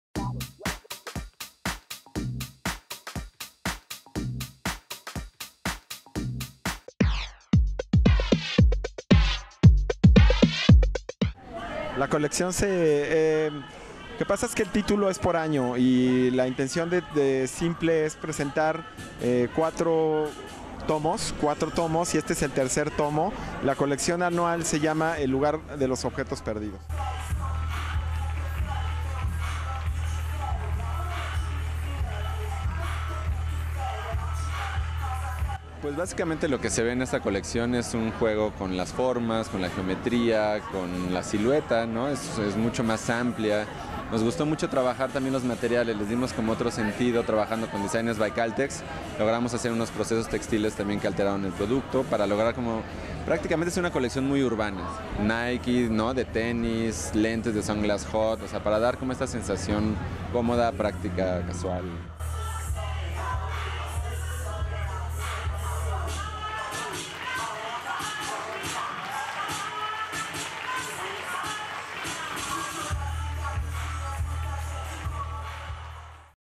Mercedes Benz Fashion Week, Diseñadores José Alfredo y Giovanni
Los diseñadores nos presentaron toda su colección para primavera/verano 2015